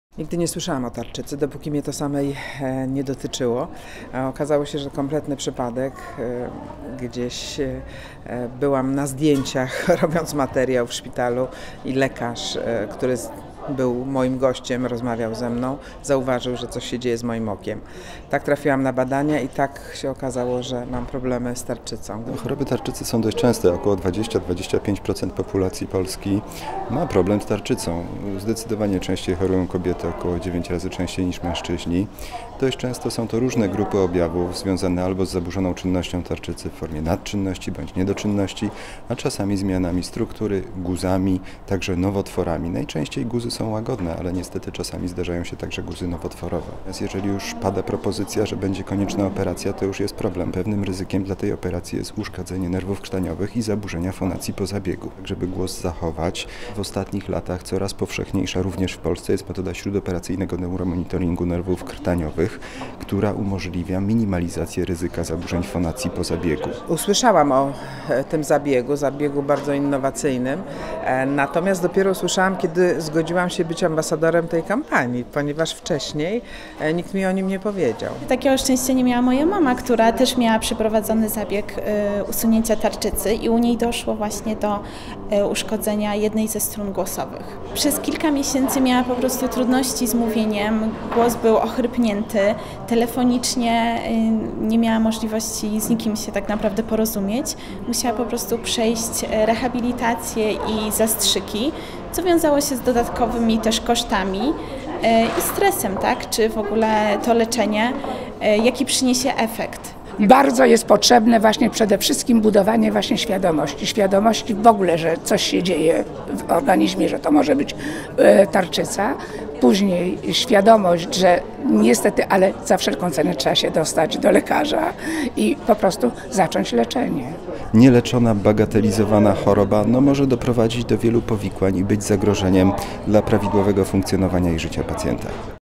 Nowoczesna metoda leczenia tarczycy pomaga ochronić głos
Wypowiedź: Katarzyna Dowbor, ambasadorka kampanii „Tarczyca – otocz ją opieką”; Prof. dr hab. n. med. Marcin Barczyński, Prezes Polskiego Klubu Chirurgii Endokrynologicznej; Katarzyna Wojtaś, pacjentka; Elżbieta Kozik, Prezes Stowarzyszenia PARS.

Choroby tarczycy są jednymi z najbardziej rozpowszechnionych chorób na świecie. W Polsce ok. 22% osób cierpi na choroby gruczołu tarczowego, bądź chorowało na nie w przeszłości. Pacjenci często obawiają się leczenia operacyjnego ze względu na możliwość wystąpienia powikłań.Jednak eksperci kampanii „Tarczyca – otocz ją opieką” podkreślają, że istnieją nowoczesne metody, które minimalizują ryzyko wystąpienia zaburzeń fonacji po operacji tarczycy.

 „Choroby tarczycy są dość częste, ok 20-25% populacji Polski ma problem z tarczycą. Zdecydowanie częściej chorują kobiety – ok. 9 razy częściej niż mężczyźni. Dość często są to różne grupy objawów, związane z zaburzoną czynnością w formie nadczynności lub niedoczynnością, a czasami zmianami struktury np. guzami, także nowotworami. Najczęściej są to guzy łagodne, ale zdarzają się także guzy nowotworowe” – przekonuje prof. dr hab. n. med. Marcin Barczyński, Prezes Polskiego Klubu Chirurgii Endokrynologicznej.

Mimo, że pacjenci zauważają wiele, często niepokojących objawów, bagatelizują je i rezygnują z wizyty u lekarza. Często też nie potrafią odczytać sygnałów, które wysyła organizm. To właśnie dlatego, duża liczba osób żyje z niezdiagnozowaną chorobą, która podstępnie niszczy ich organizm.

„Nigdy nie słyszałam o tarczycy, dopóki choroba nie dotknęła mnie osobiście. Wykryto ją przez przypadek” – podkreśla Katarzyna Dowbor, Ambasadorka Kampanii.

Okazuje się, że ok. 25% populacji ma jakieś schorzenie tarczycy, w którym są zmiany guzkowe (m.in. nowotworowe). W tej grupie pacjentów należy bardziej wnikliwie zastanowić się nad leczeniem operacyjnym. Operacja gruczołu tarczycy może być również konieczna, gdy pacjent posiada duże wole tarczycowe, uciskające drogi oddechowe lub powodujące niekorzystny efekt kosmetyczny nawet przy zachowaniu prawidłowej funkcji tarczycy. Zabieg ten wskazany jest również przy nadczynności tarczycy oraz obecności nowotworu złośliwego lub podejrzanych guzków. Decyzja o usunięciu chorego gruczołu zawsze poprzedzona jest szeregiem badań.

„W przypadku leczenia operacyjnego istnieje ryzyko uszkodzenia nerwów krtaniowych i zaburzenia fonacji po zabiegu. W celu zachowania głosu po zabiegu, w ostatnich latach coraz popularniejsza jest metoda śródoperacyjnego neuromonitoringu nerwów krtaniowych, która umożliwia minimalizację ryzyka zaburzeń fonacji po zabiegu” – przekonuje prof. dr hab. n. med. Marcin Barczyński, Prezes Polskiego Klubu Chirurgii Endokrynologicznej.

Według szacunków Polskiego Klubu Chirurgii Endokrynologicznej metoda neuromonitoringu nerwów krtaniowych jest stosowana w blisko 100 oddziałach chirurgicznych w Polsce i około 20% spośród 25 tysięcy operacji tarczycy wykonywanych rocznie w naszym kraju jest w ten sposób monitorowanych (stan na grudzień 2017 r.).

Ogólnopolska Kampania Edukacyjna „Tarczyca – otocz ją opieką”, organizowana przez Stowarzyszenie Polskie Amazonki Ruch Społeczny (PARS), ma na celu  podniesienie poziomu wiedzy na temat chorób tarczycy, ich diagnozowania oraz profilaktyki zarówno w terapiach farmakologicznych, jak i najnowszych możliwościach chirurgii endokrynologicznej.